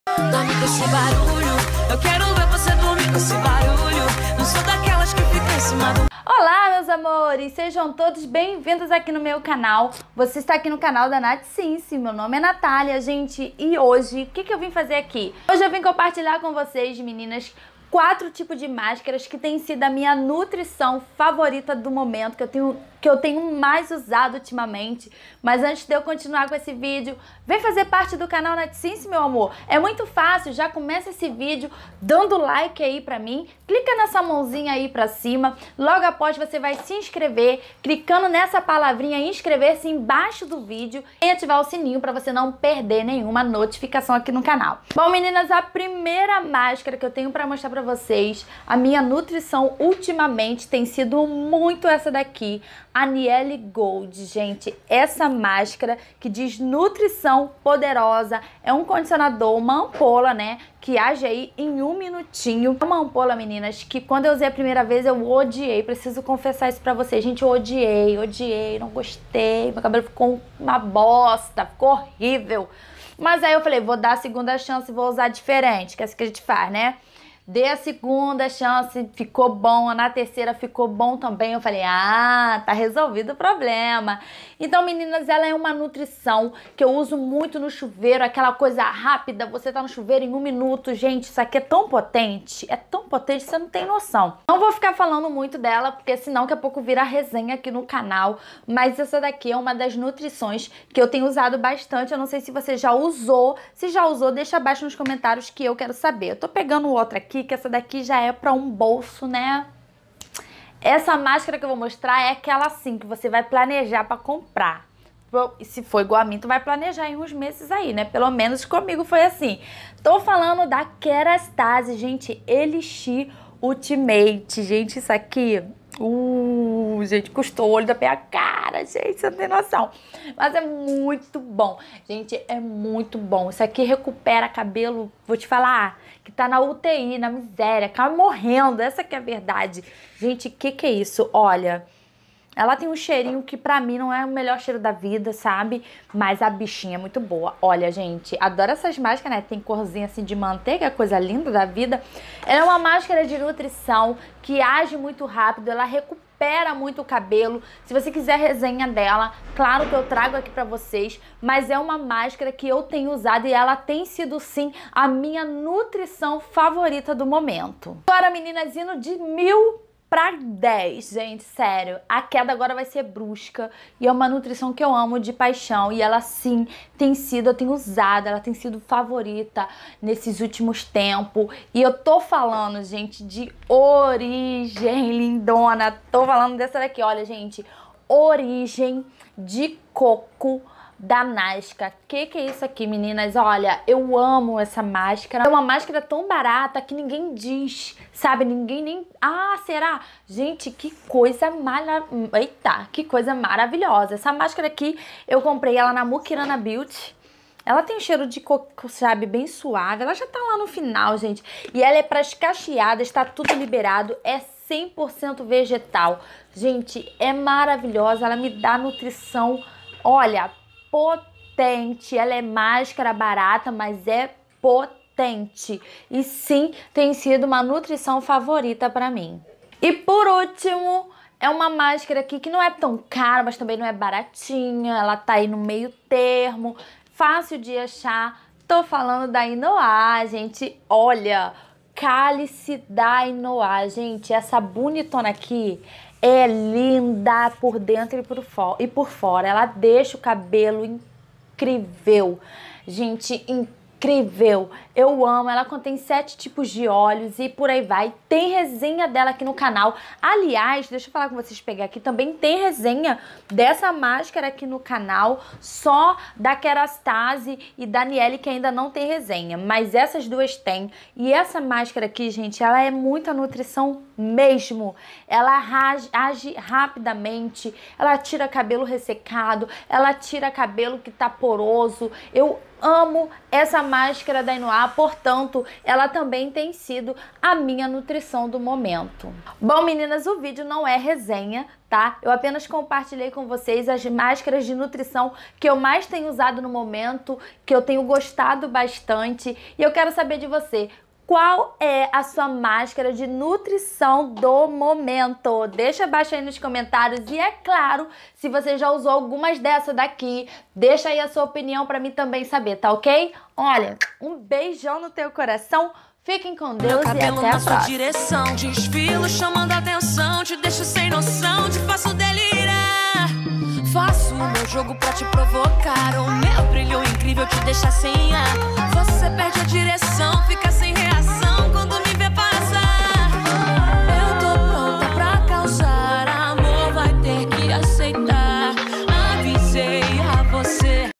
AS MELHORES NUTRIÇÕES DO MOMENTO / TOP /FAVORITAS
Lindonas e ai vamos conferir as nutrições que tem feito meu momento capilar mais feliz!!!
    A etapa de nutrição é muito importante pra mim pois ela salva cabelos que estão extremamente ruim.. Por tanto vou te dar dicas de nutrição que sei que vc vai amar!!!

Contato pra parcerias:
Cep: 26086215 /n: 568 /casa 2 andar